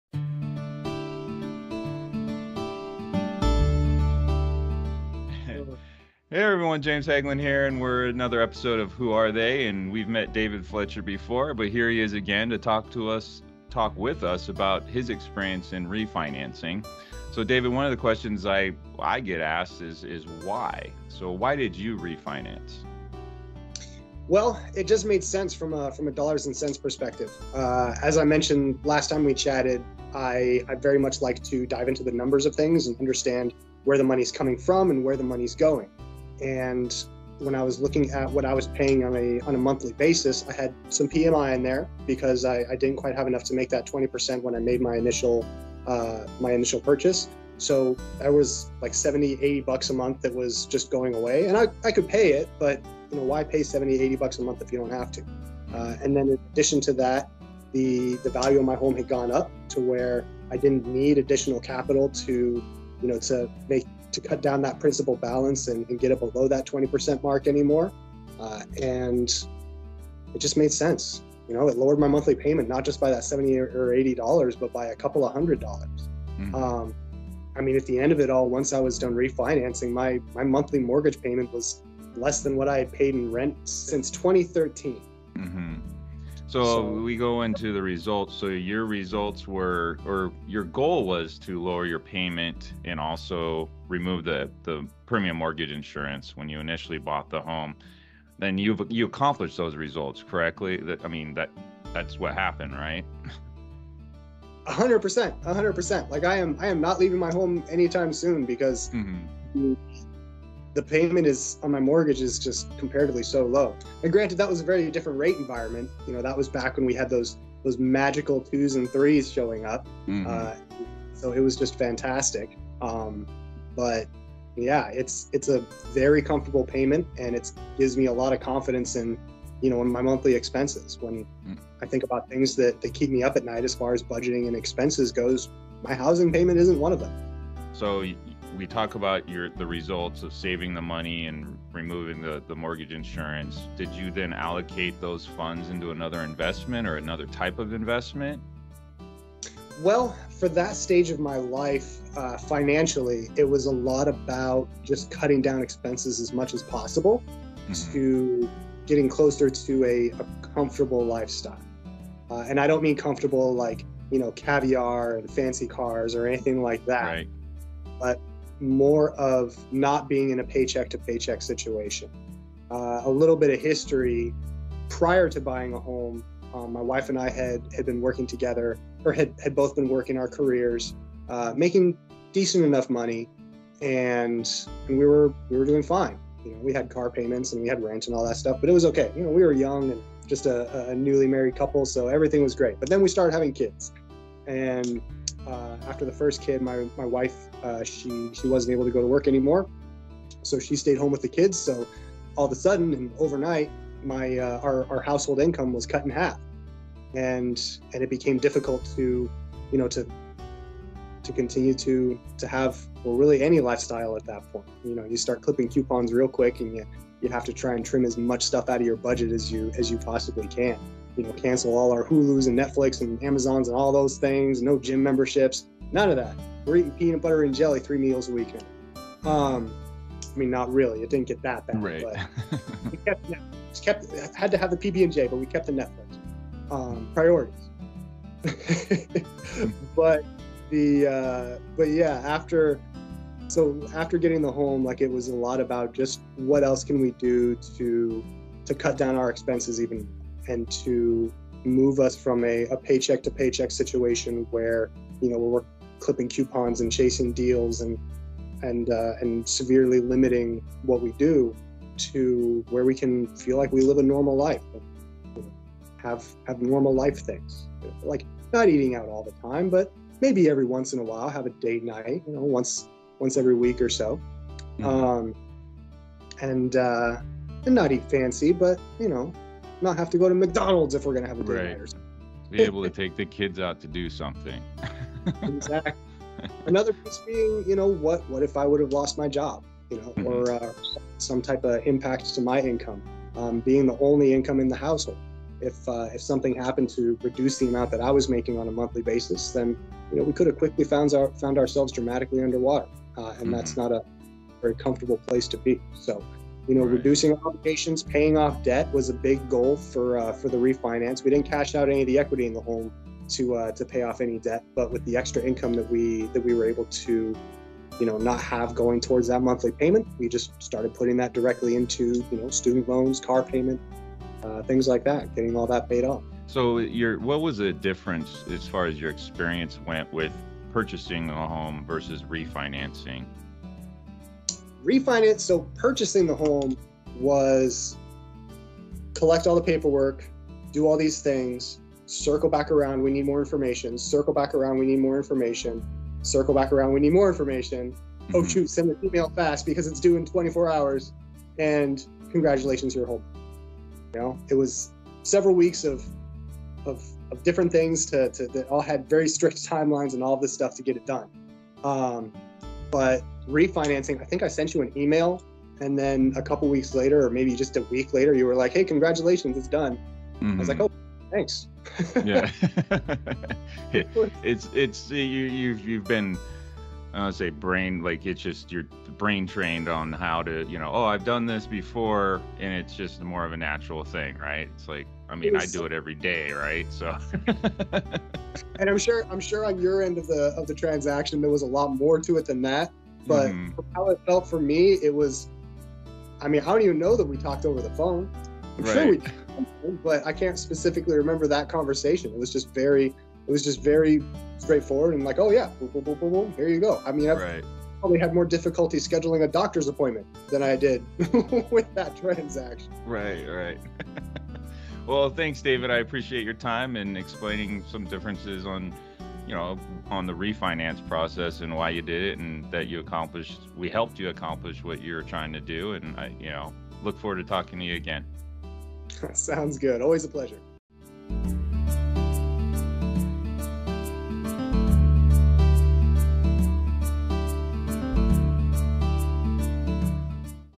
Why did David Refinance?
David shares with us Why they Refinanced their mortgage, the results and the experience.

Daniel Kim - Real Estate agent at Daily Realty Group, LLC